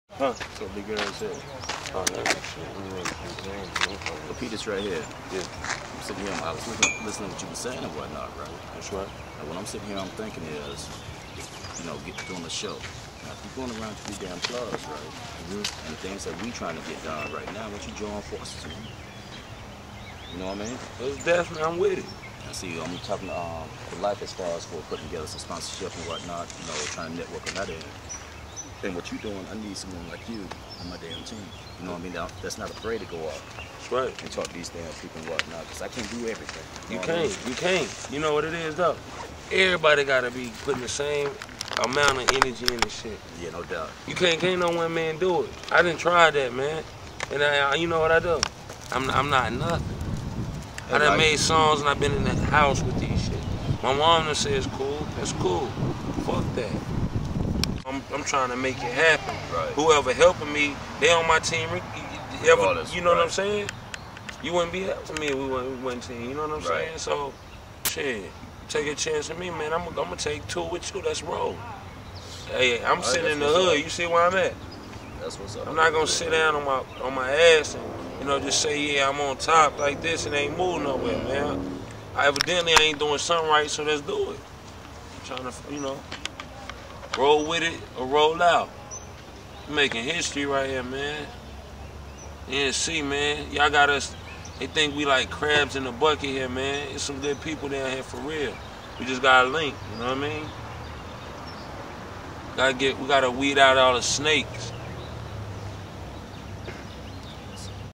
Hip Hop Elevation Prodz. Archives South Mount Park Greensboro, NC/May 28, 2005
Elevation Productionz Archives
South Mount Park
Greensboro, NC
May 28, 2005
Footage By: Supreme Life aka Grand Daddy Frost